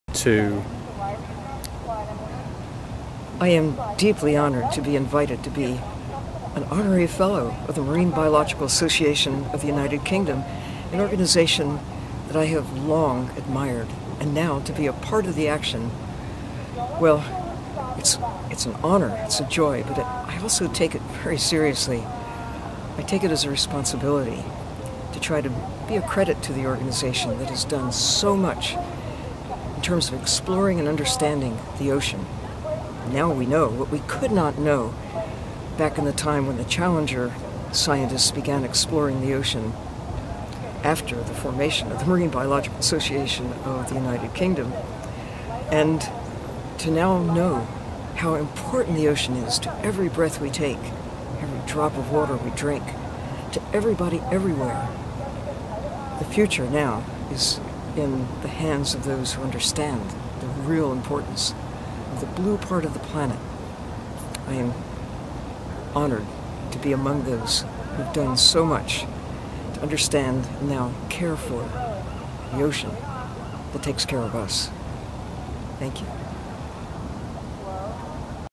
Sylvia Earle MVI 2305
In 2014, Sylvia Earle was invited to be an Honorary Fellow of the Marine Biological Association of the United Kingdom. This is a short film of her acceptance speech.